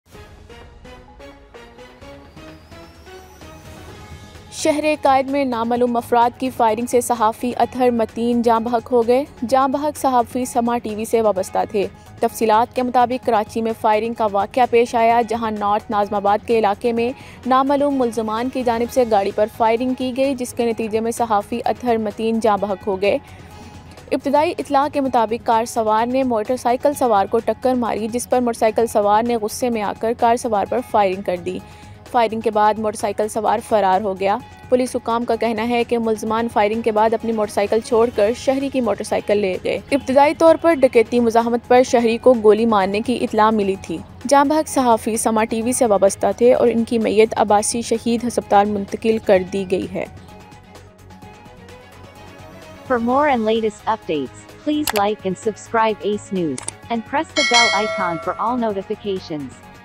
Breaking News - Dakaiti muzamat per Ather Mateen Ahmed Senior Producer SAMAA TV Janbahaq
ناظم آباد میں کار پر فائرنگ سے سماء ٹی وی کے سینئر پروڈیوسر اطہر متین جاں بحق ہوگئے۔